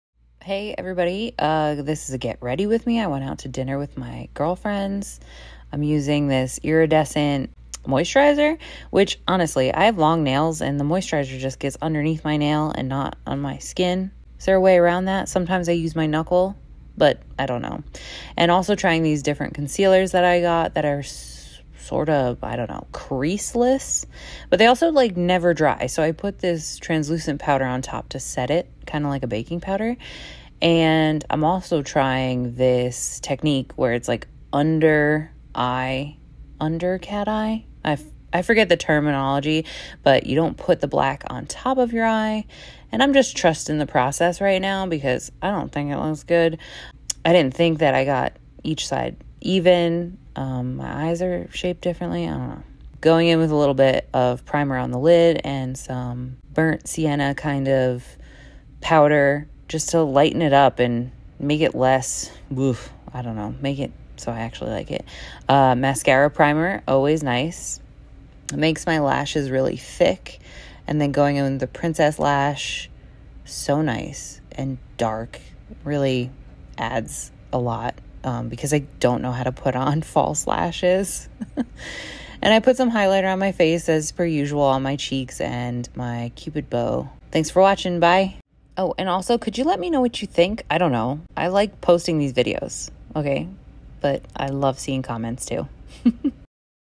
Reverse Cat Eye Makeup Look
Get ready with me!! Cat eye but reversed?! Under eyeliner woo feeling cute and kinda Egyptian influenced rn. If only my hair was it’s natural dark color very cleopatra. What do you think?

Pls ask if you ever wana know what Products I’m using 😉